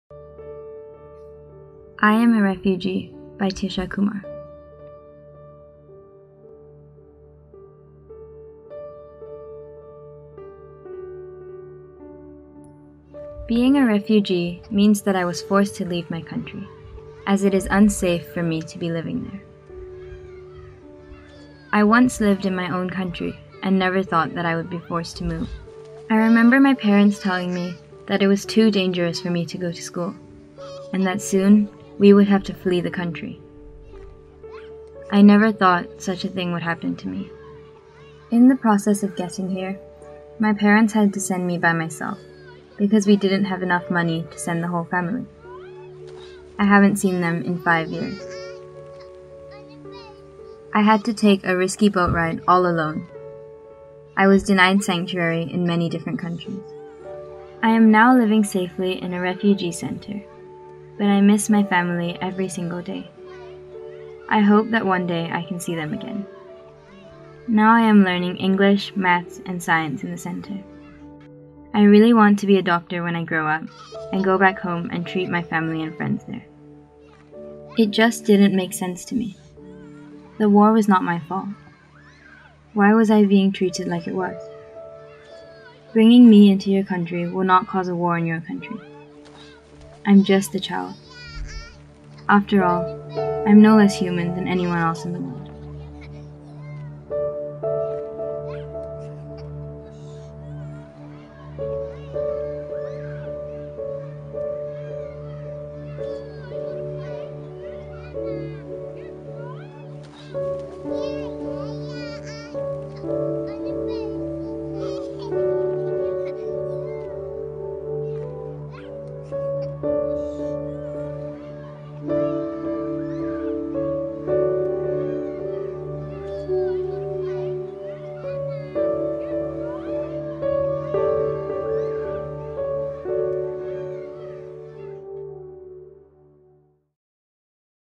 GIS | I Am A Refugee
I am A Refugee is a book by GIS student, Tishya Kumar, which shares the experiences of young Rohingya refugees living in Malaysia. The video is a recording of the author's book reading and original artwork from the book.